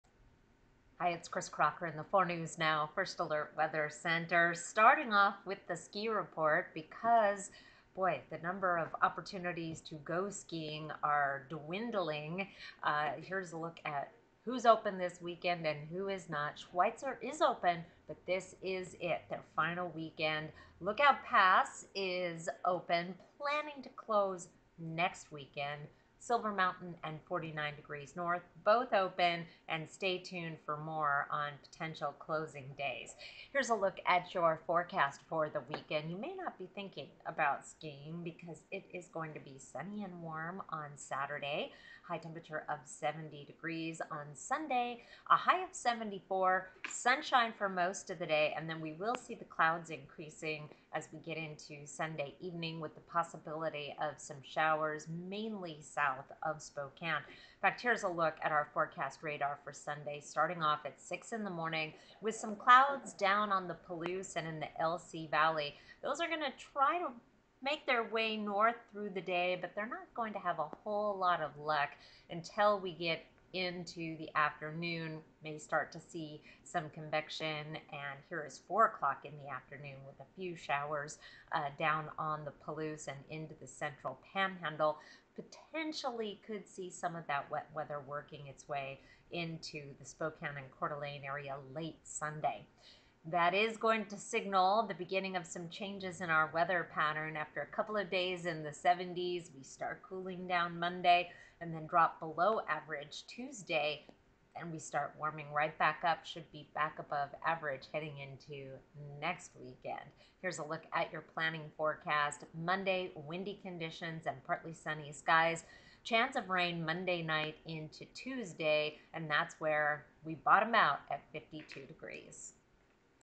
Warm, sunny weekend followed by some changes next week
First Alert Forecast